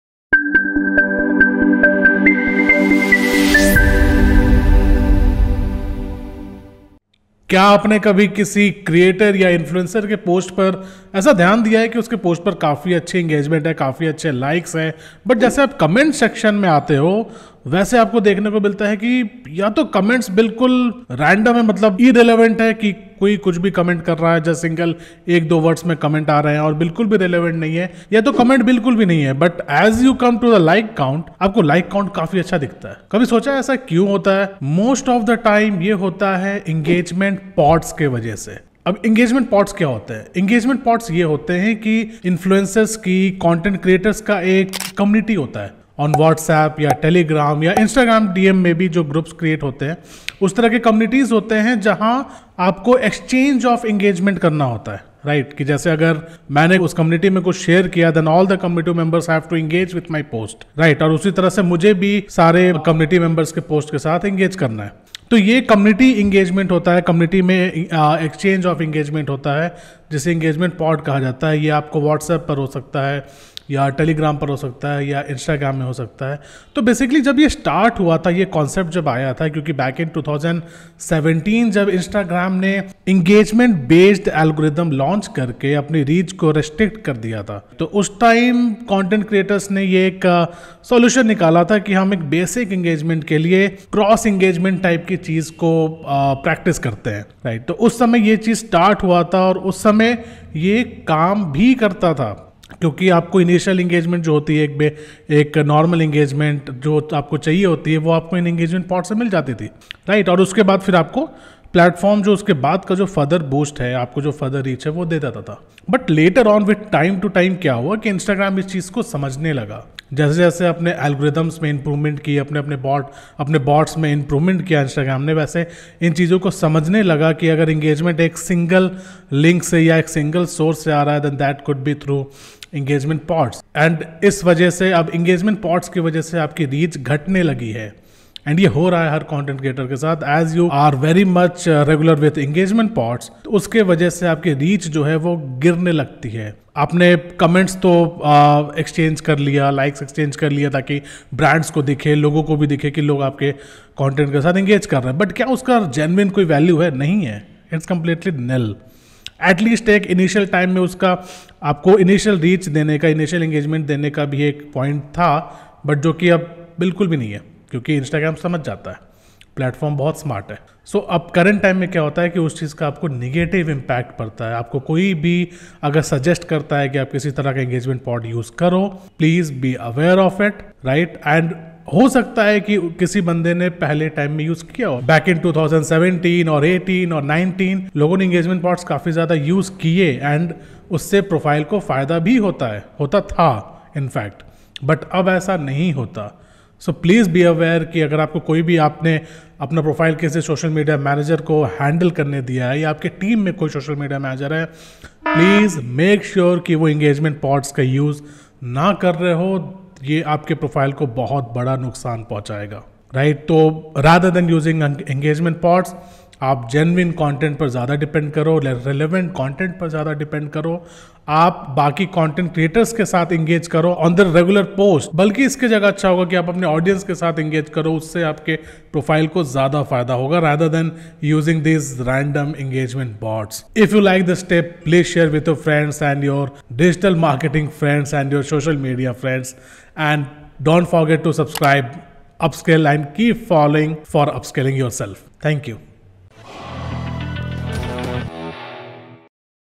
Fake Engagement On Instagram ‼️
Doston aaj ke video me hum log jaanenge ke kaise fake engagement pods aapke profile pr negative impact daalte hai.

Hope this will be a useful video for you all.